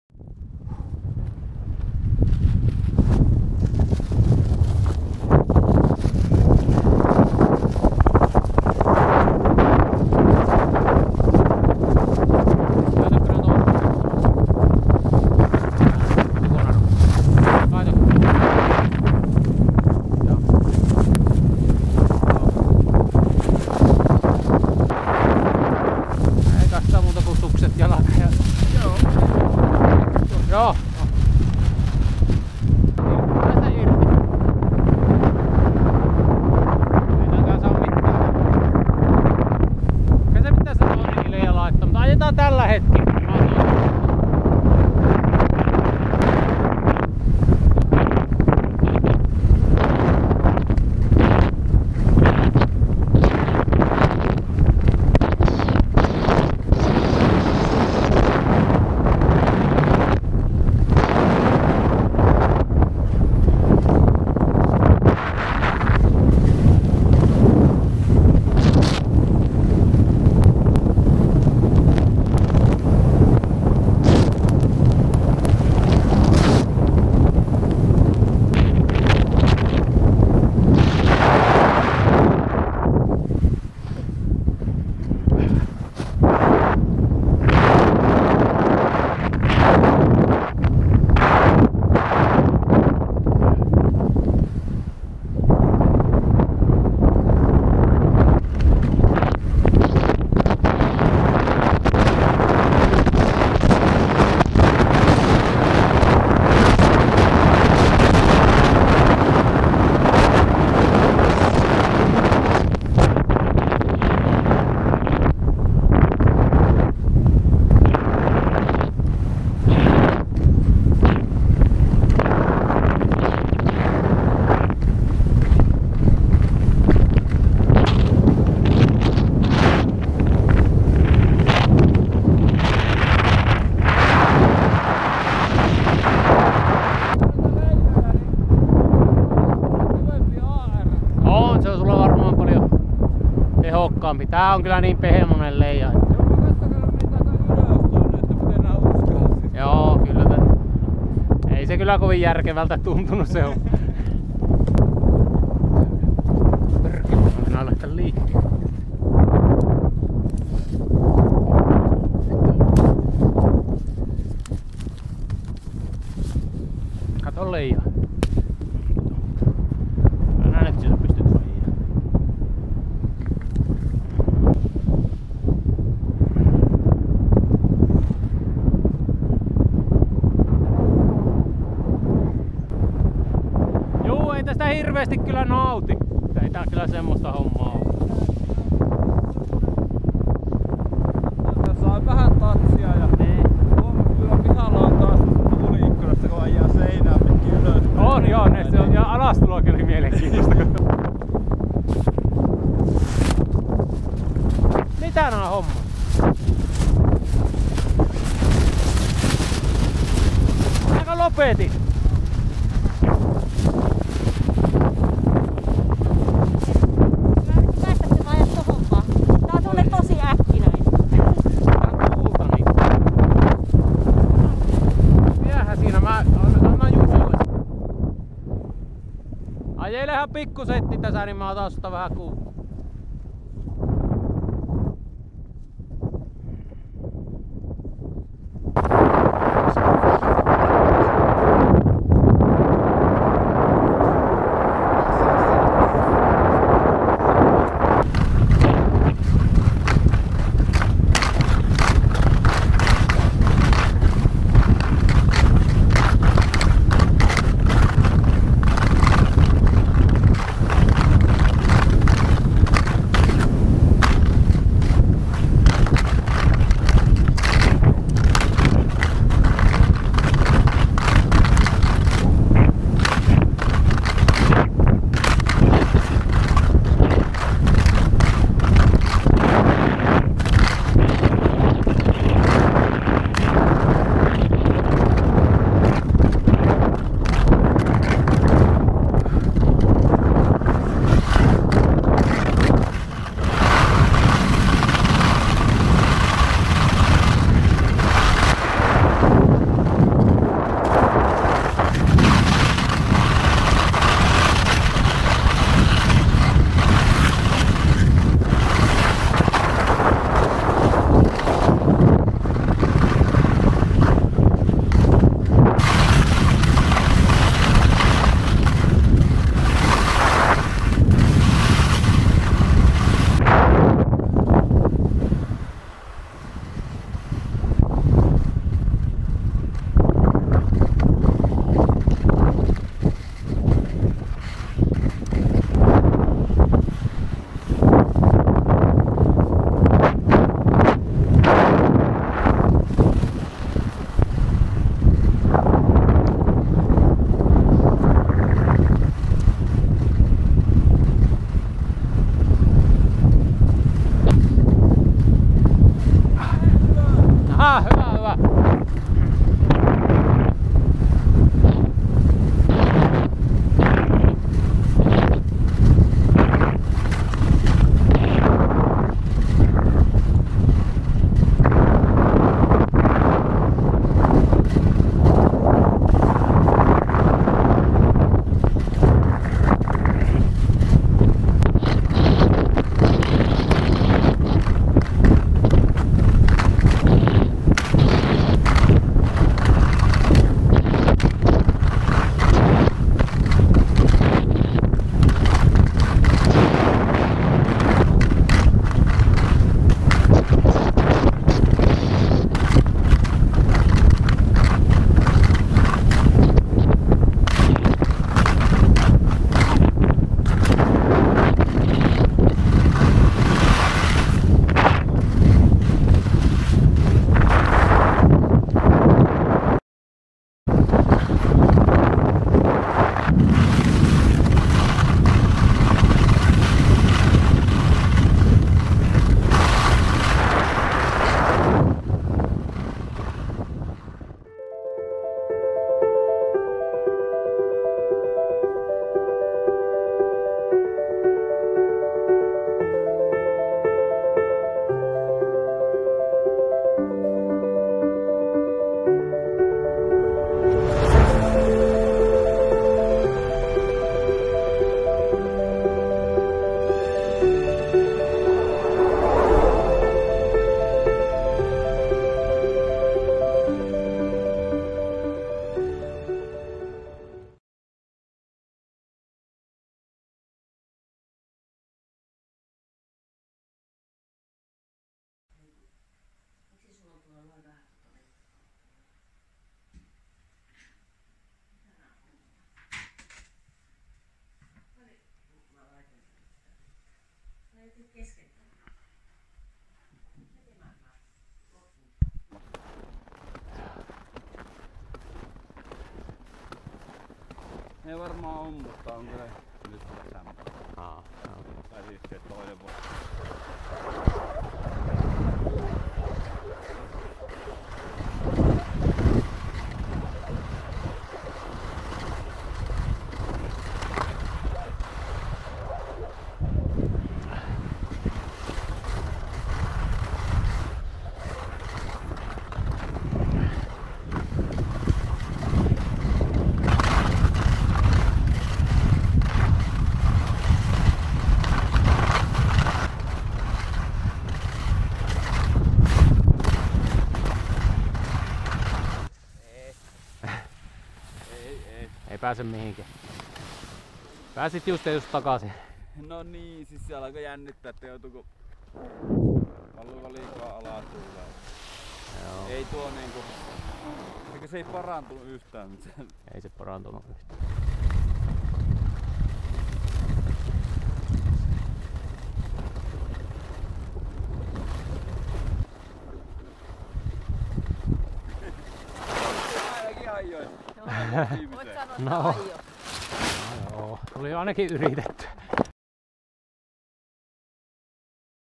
Kite Weekend in Pallas. Early April of 2023
Kite Skiing in Pallas area in Lapland. Varying winds from weak to very strong